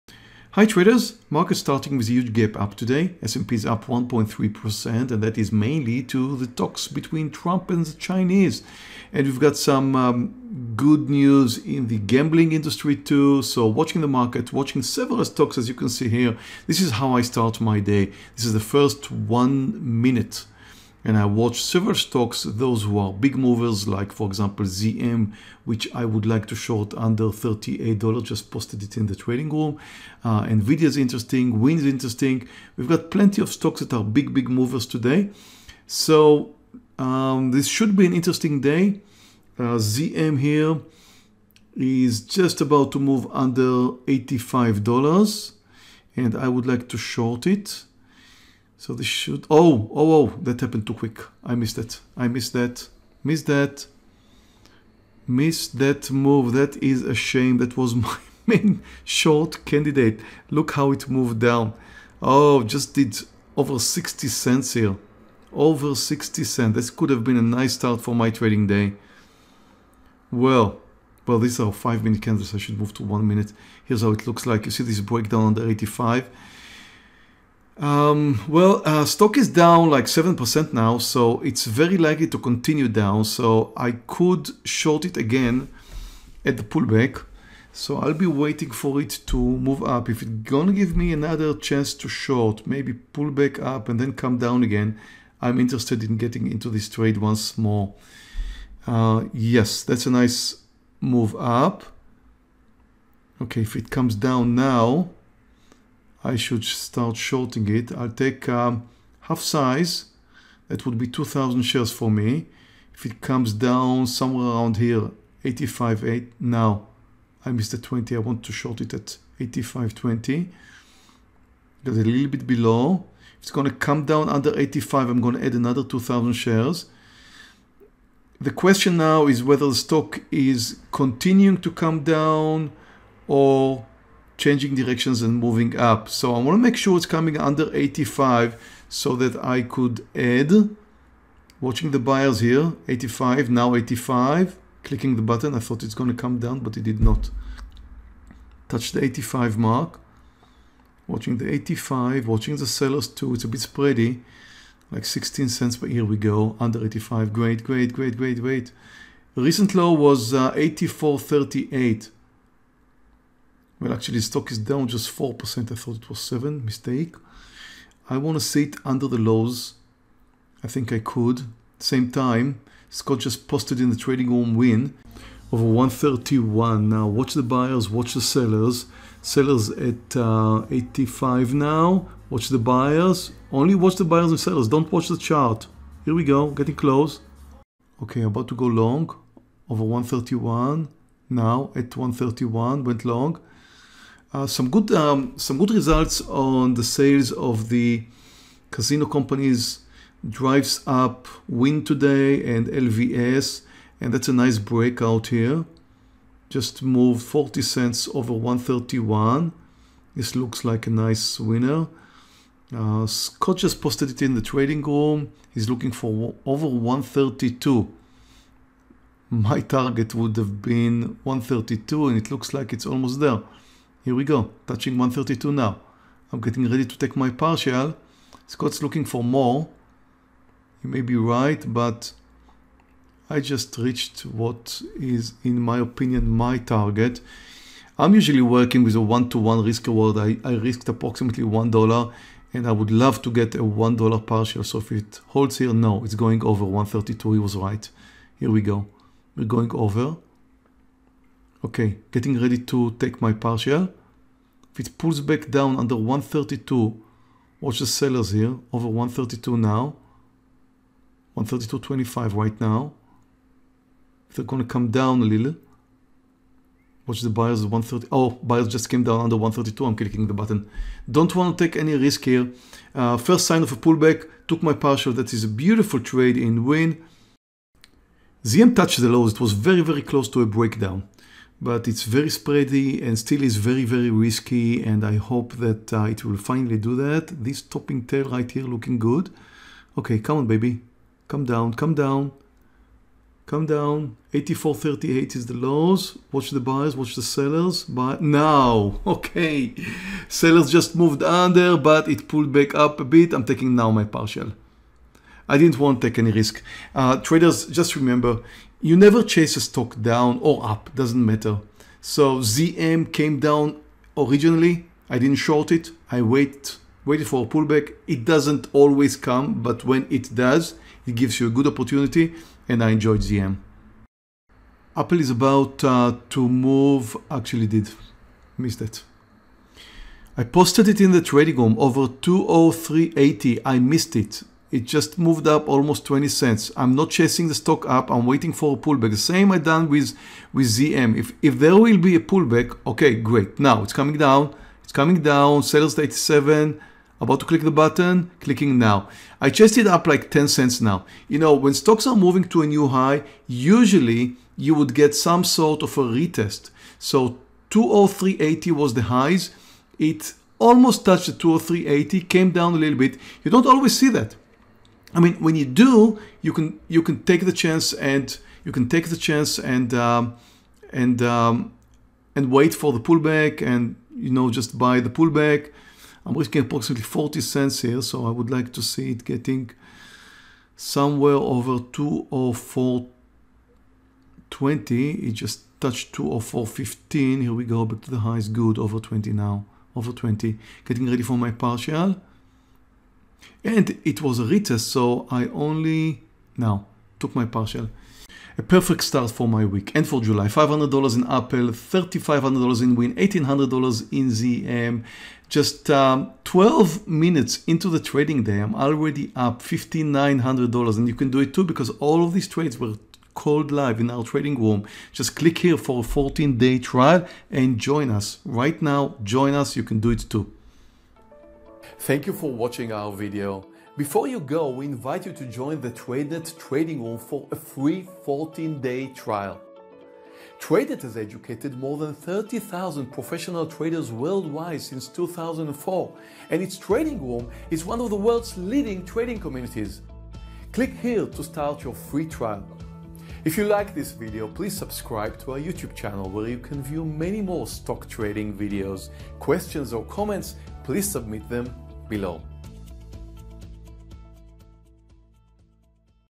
Live Day Trading Breakouts For $5,000 in 12 minutes - Meir Barak
Live Day Trading Stocks - traded two breakouts and one breakdown for three green trades and a great start for July for a profit of $5,000 in only 12 minutes. Done for the day! Meir Barak day trading live on July 1st. 2019.